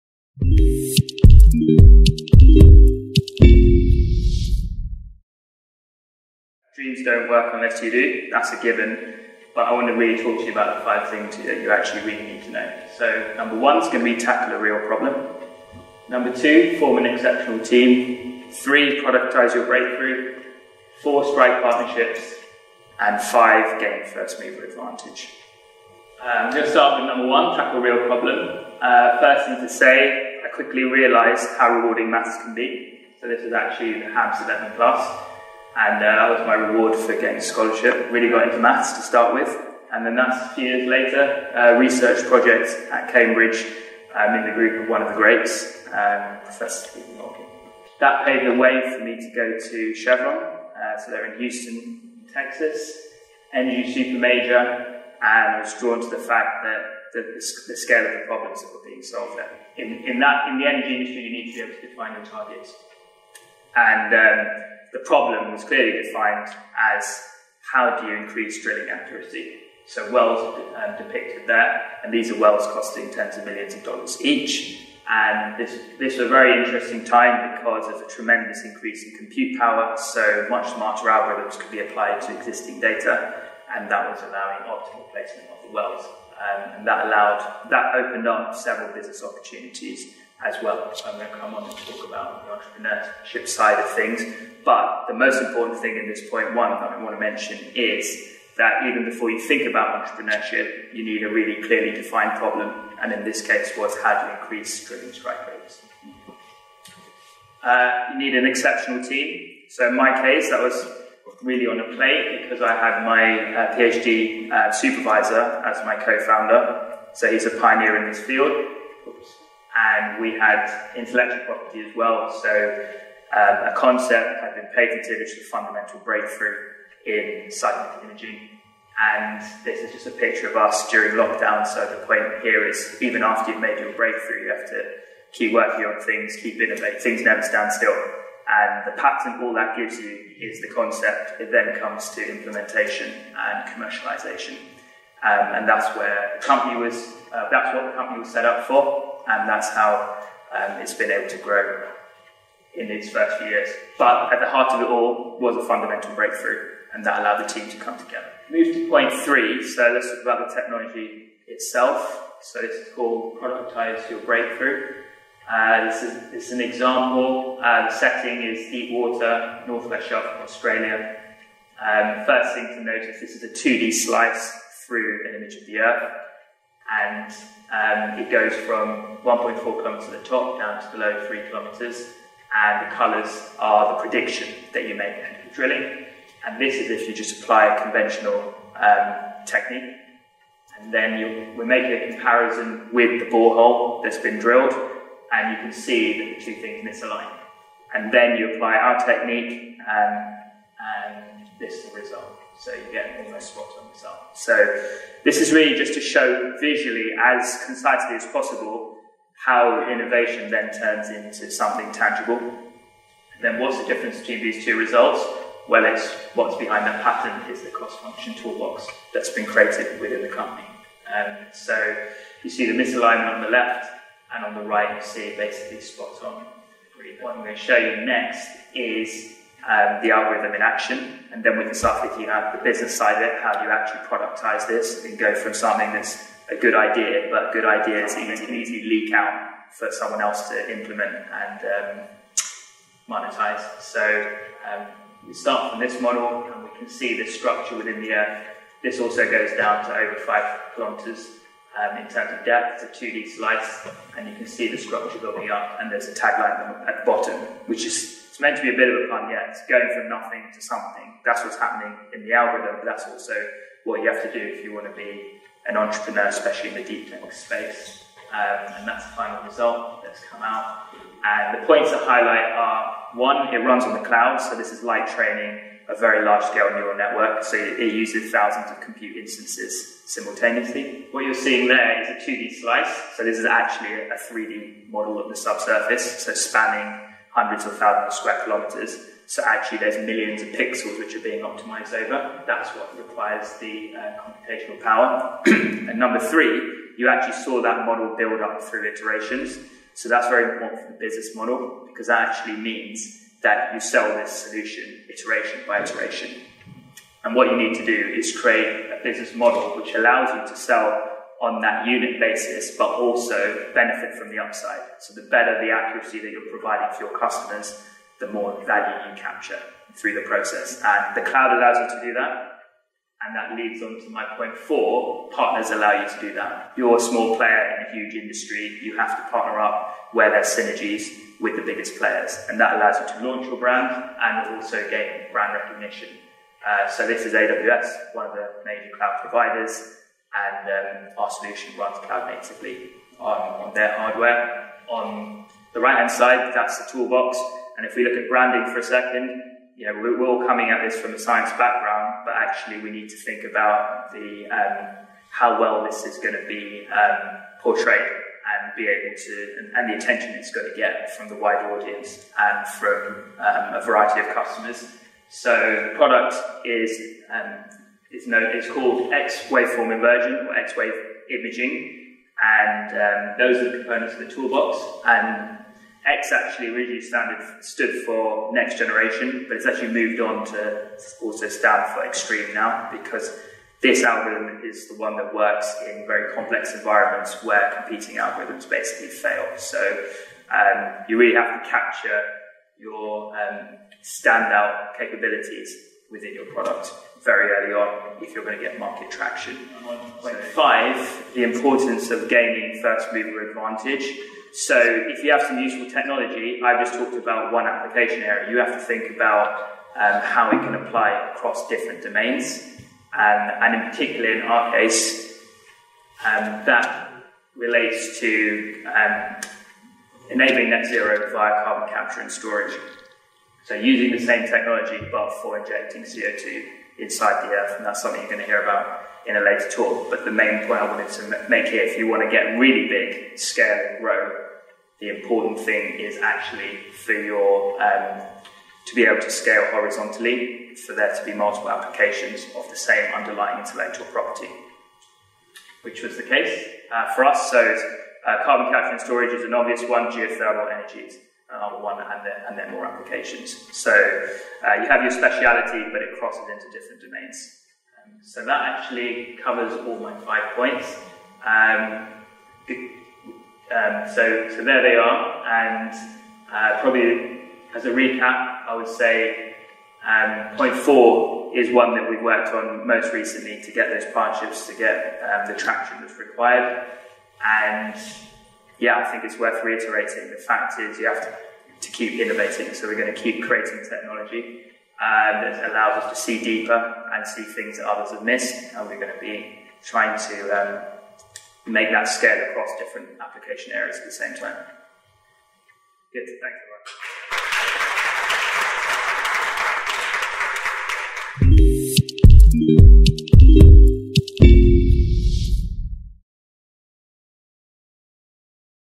Deep Tech Entrepreneurship - 5 things you need to know | Nikhil Shah | TEDxYouth@HABS
This talk by Dr Shah covers the key steps for Entrepreneurship, linking in his own personal journey to become the CEO of S-Cube.  Nikhil Shah studied Maths at Cambridge and a PhD at Imperial College. He co-founded Imperial spinout S-Cube in 2019 with his doctorate supervisor (Prof. Mike Warner). He was at Habs from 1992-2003 where his interest in maths, technology and entrepreneurship began. This talk was given at a TEDx event using the TED conference format but independently organized by a local community.